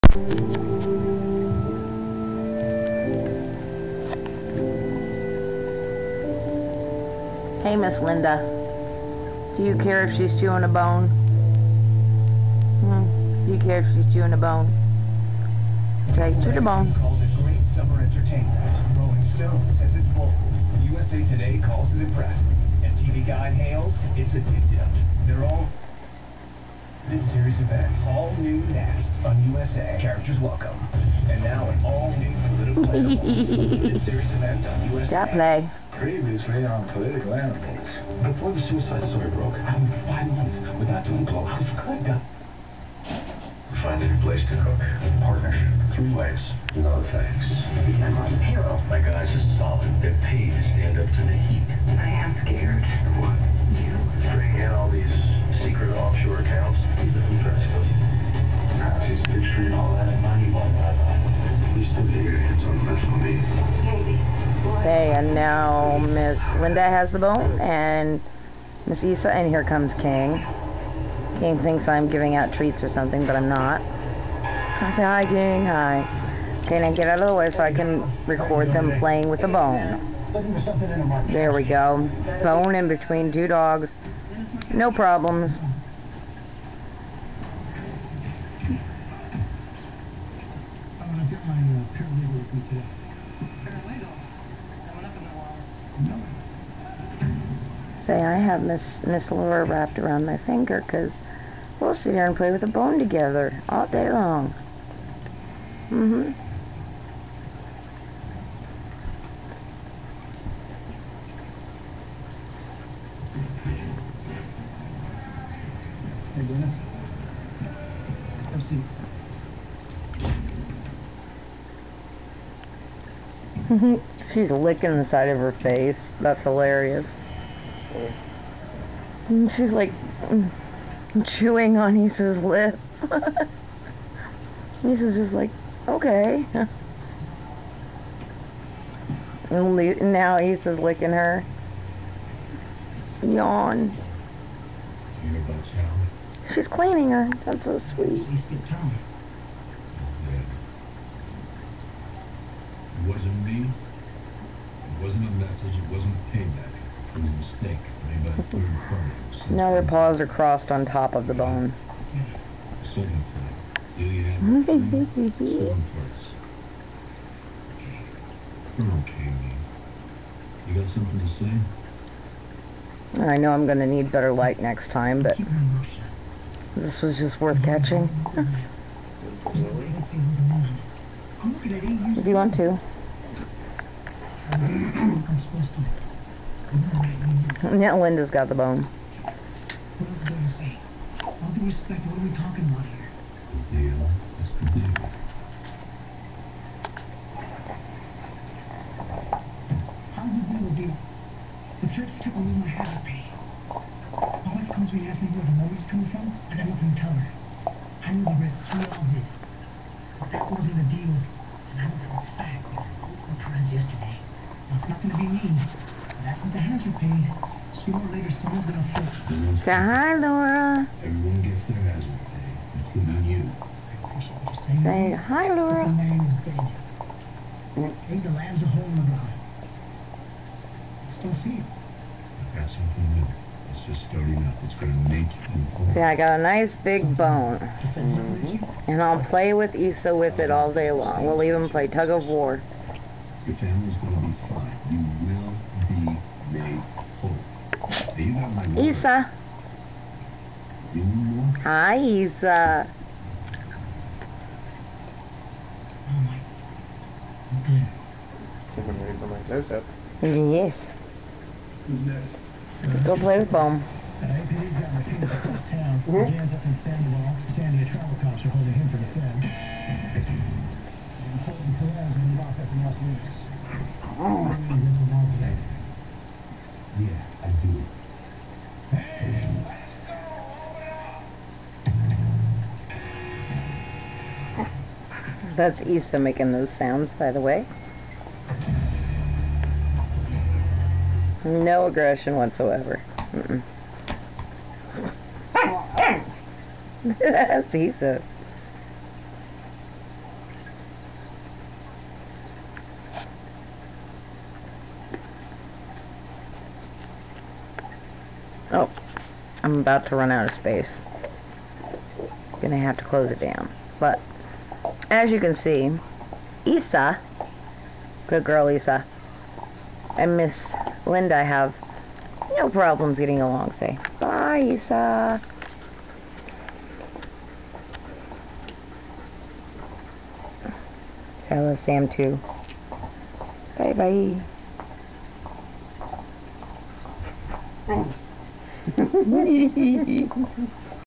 Linda around other dogs - PART 2 (bone chewing time!)
Just a video of what we see all the time, and how well she does with other dogs - Linda has no aggression in the videos whatsoever!  She is happily chewing away at a bone, and all barking and growling comes from the pointy-eared puppy Isa who is playing, and trying to get Linda's bone.  Note Linda's lack of aggression in response!  For those watching, Linda is a Basenji Shepherd mix available for adoption through